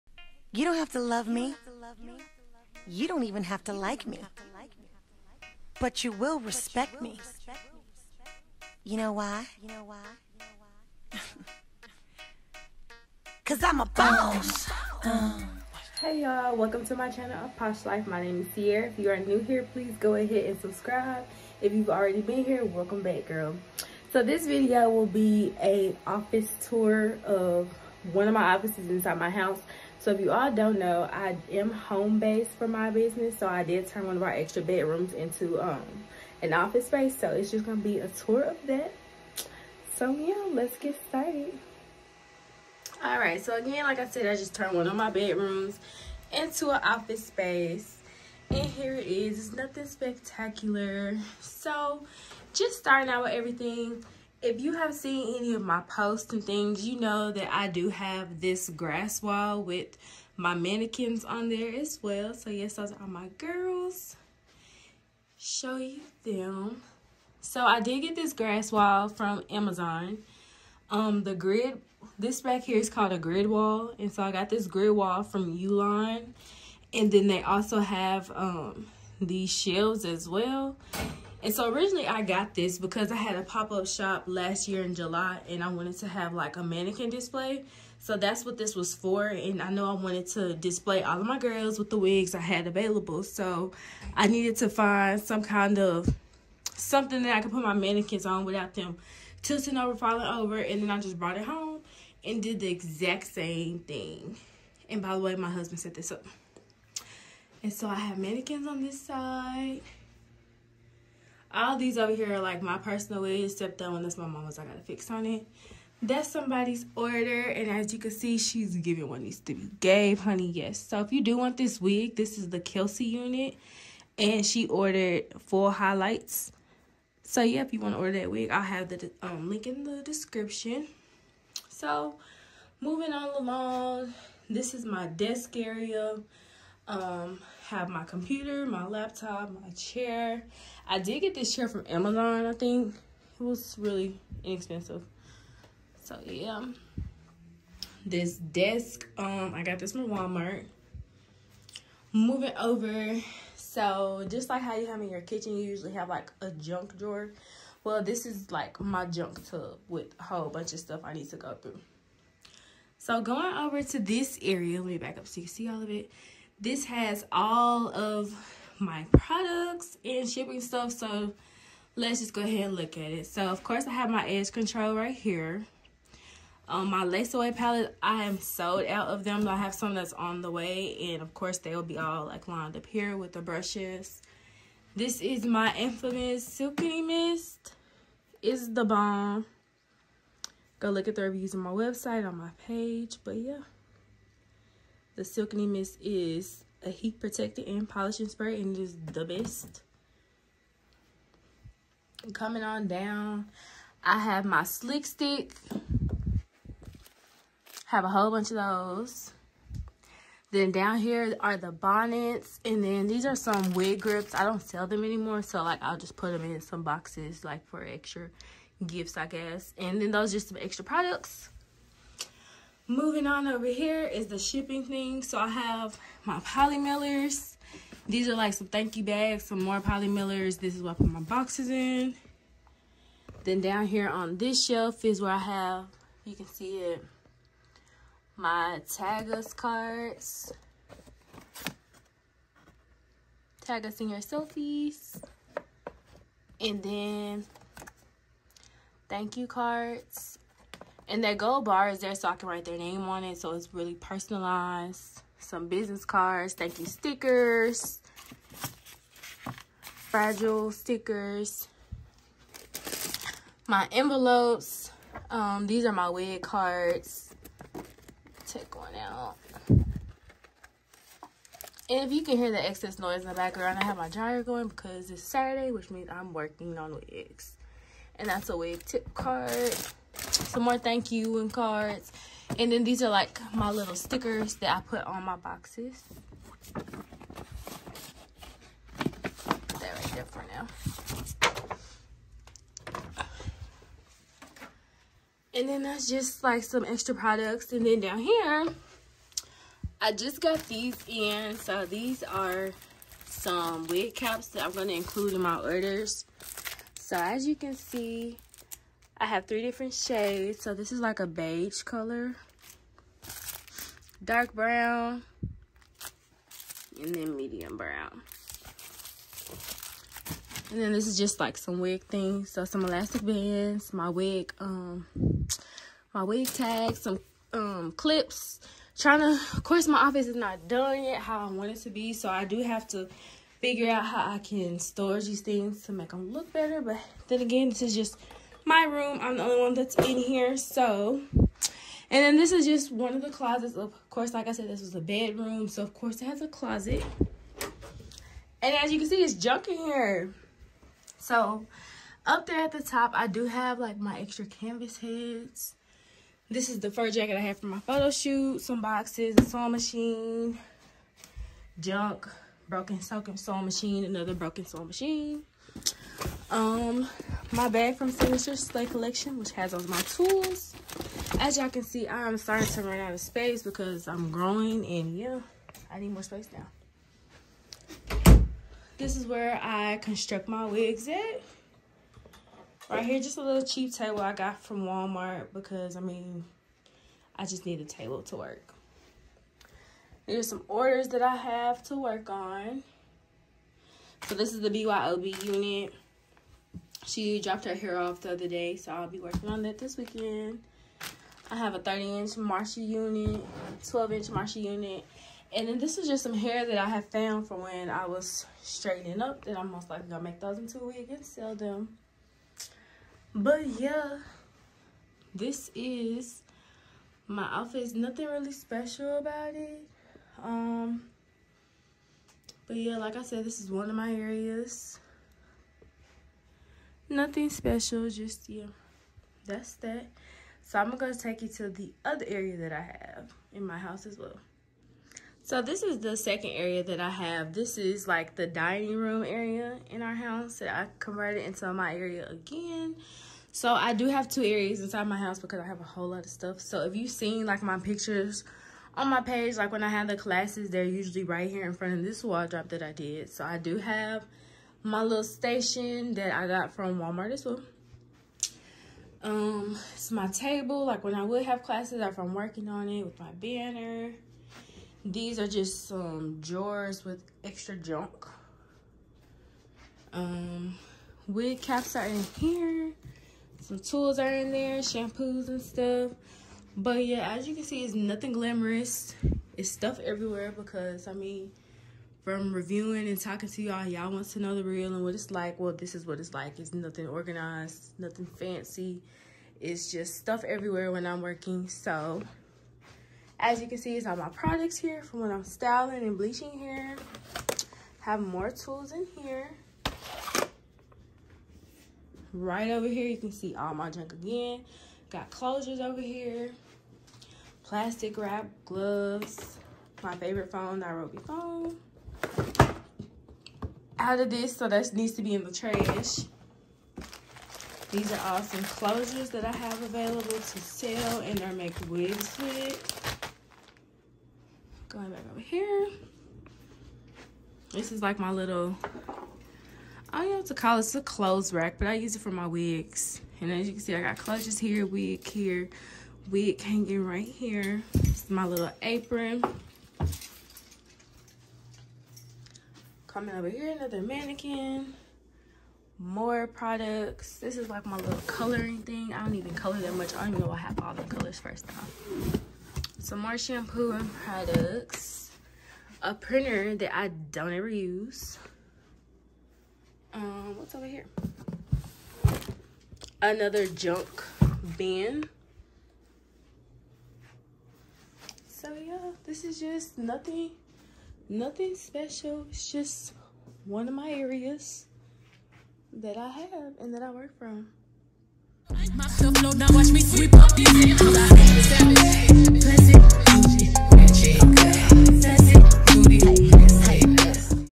DIY GRASS WALL | WIG ROOM | HOME OFFICE TOUR
Welcome to my channel “A POSH Life” if youre new here Welcome and if you’v been here before Welcome Back💗 This video is a tour of my home office/wig rooms and how I have everything organized or not so organized😅😂 the description also list where you can find the things shown.

💇🏾‍♀️Interested in purchasing the wig shown?
26” Kelsi Unit with Full Highlights (Add on)

Grass Wall

Office Items⤵️
L- Shape Desk
Book Shelf(used to hold products): Mainstays 71" 5-Shelf Bookcase with Adjustable Shelves, White
3D Wall Art(pink and silver back drop)